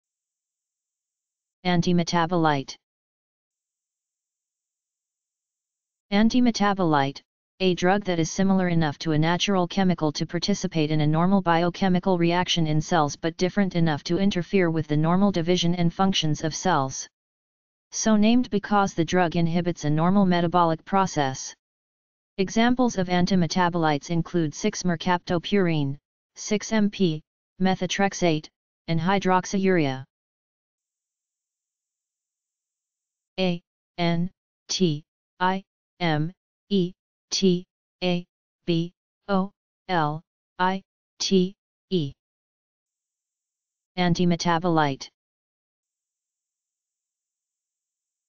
Antimetabolite - Medical Definition
Antimetabolite

Antimetabolite: A drug that is similar enough to a natural chemical to participate in a normal biochemical reaction in cells but different enough to interfere with the normal division and functions of cells. So named because the drug inhibits a normal metabolic process. Examples of antimetabolites include 6-mercaptopurine (6MP), methotrexate, and hydroxyurea.